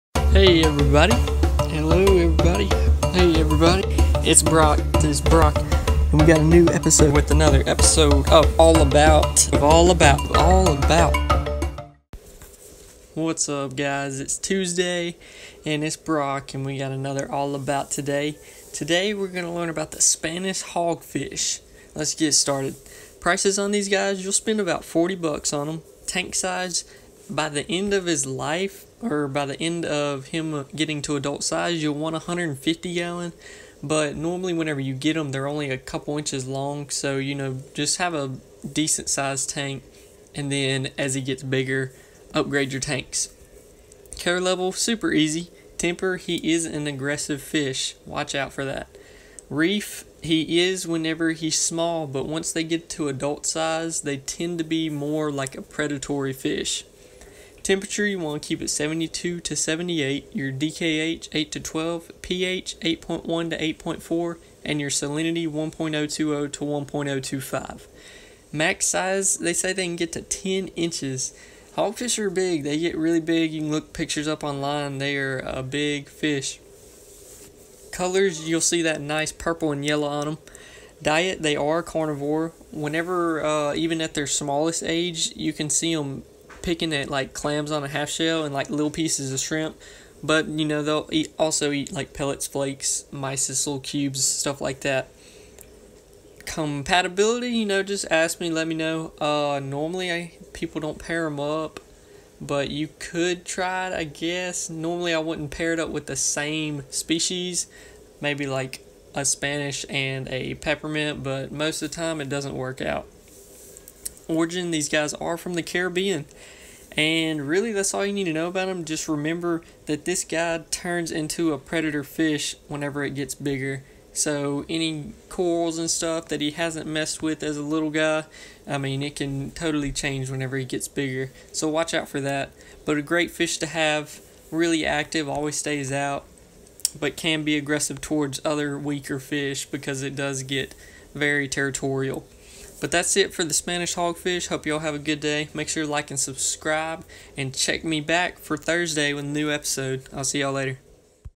All About The Spanish Hogfish
An LSU fish where I am from. A great fish with a great personality that will stay out and hang with everybody. Tends to become more predatory once it reaches its adult age.